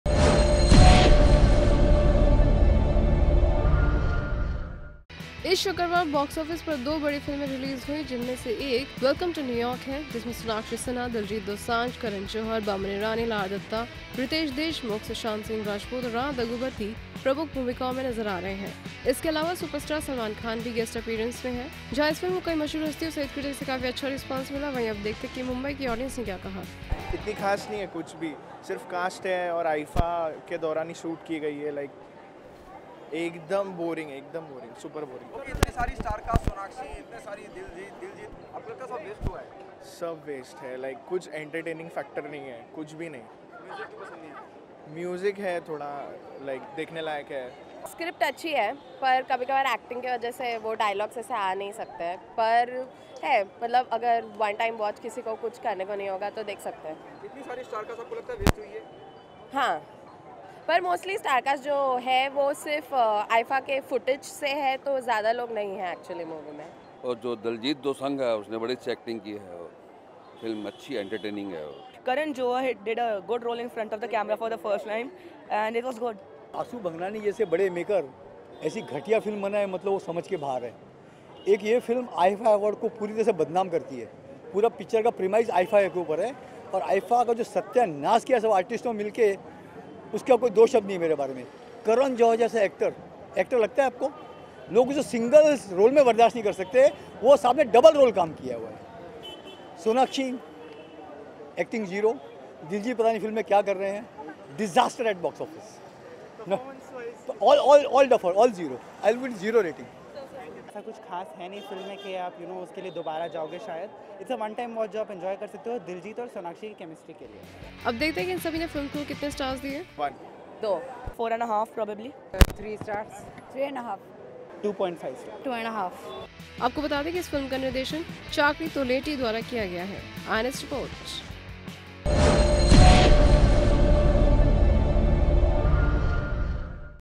' Welcome to New York ' | Public REVIEW
इस शुक्रवार बॉक्स ऑफिस पर दो बड़ी फिल्में रिलीज हुई हैं जिनमें से एक 'वेलकम टू न्यूयॉर्क'  हैं ,  जिसमें सोनाक्षी सिन्हा,दिलजीत दोसांझ,  करण जौहर, बमन ईरानी, लारा दत्ता,रीतेश देशमुख,  सुशांत सिंह राजपूत और राणा दग्गुबाती प्रमुख भूमिकाओं नजर आ रहे हैं. इसके अलावा, सुपरस्टार सलमान खान भी गेस्ट अपीयरेंस में हैं.